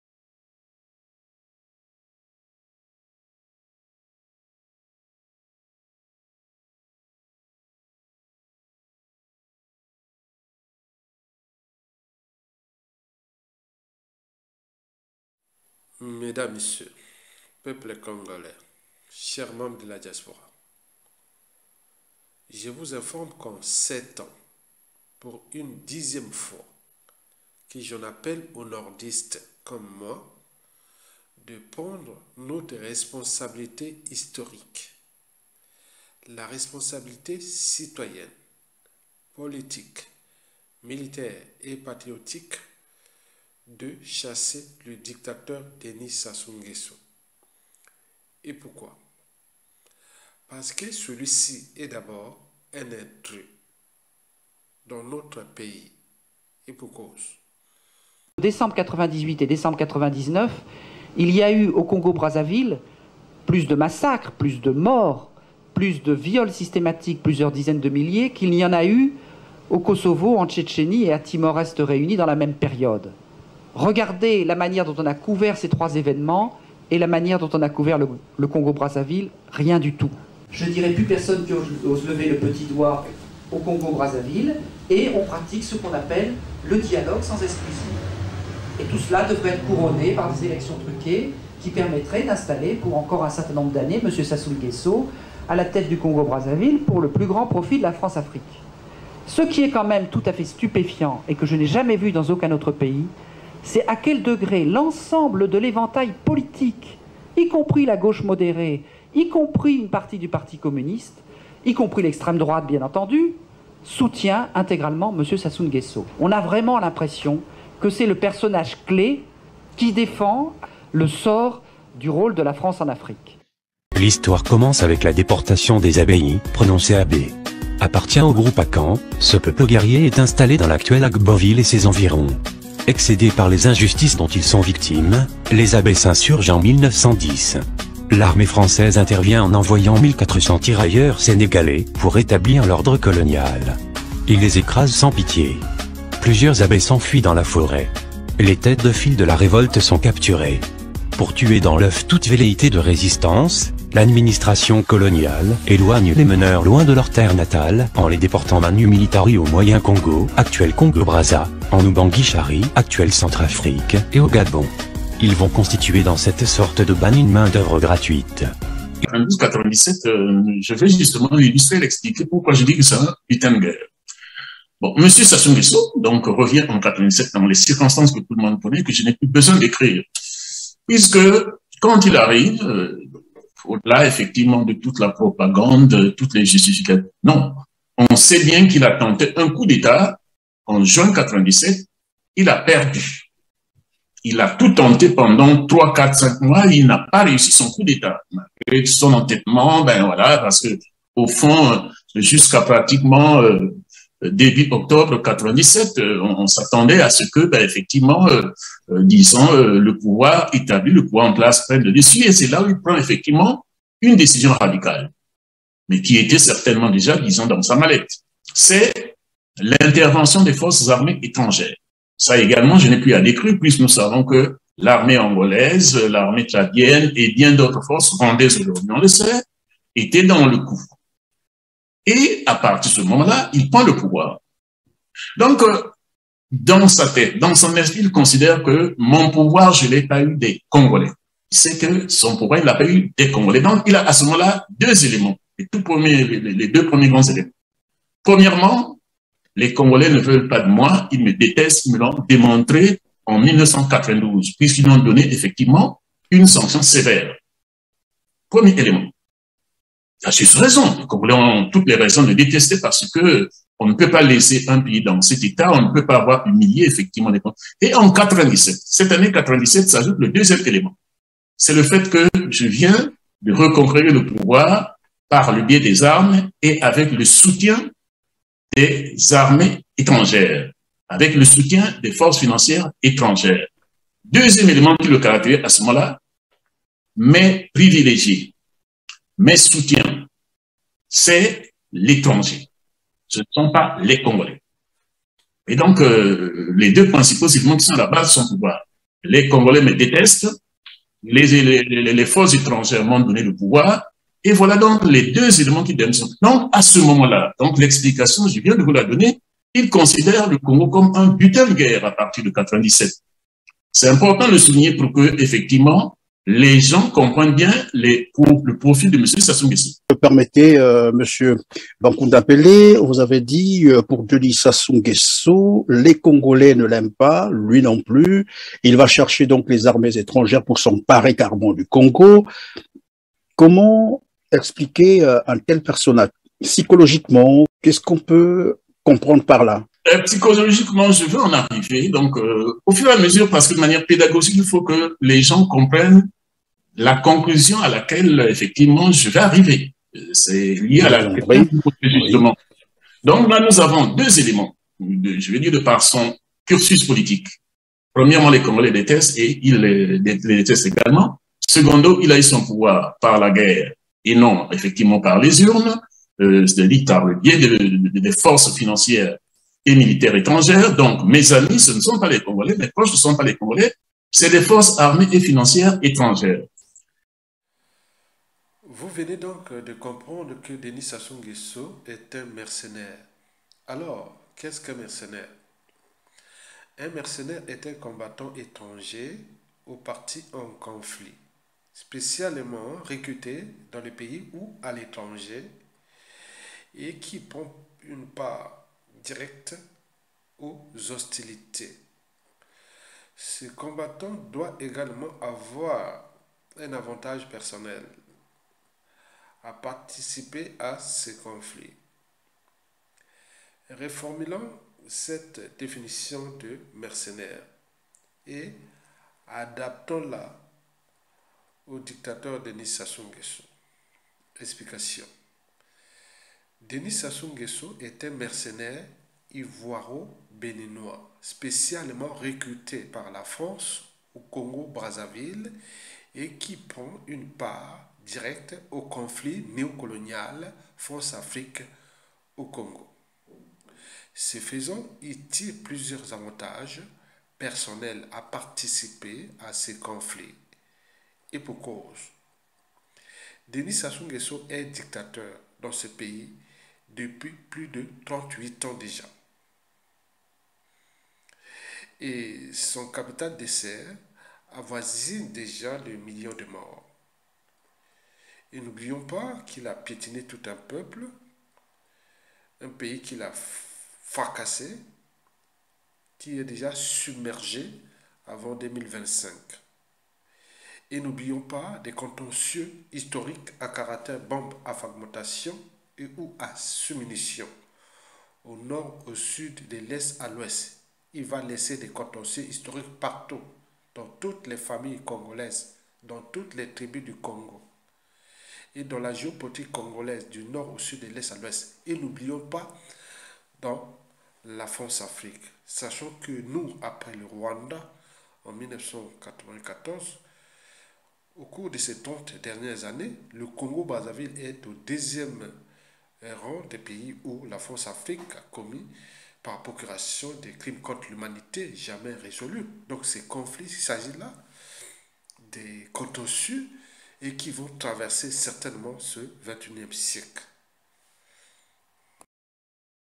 LA SANTÉ DE SASSOU SE DÉGRADE EN TURQUIE ET QU'EST CE QUE CA PEUT FAIRE? NORDISTES LIBÉRONS LE PAYS.
Pour contribuer à la lutte contre le régime de Brazzaville, soutenez la résistance
L'après Denis Sassou Nguesso qui se dessine à nos yeux est dans le passé et le présent qu'on ignorent.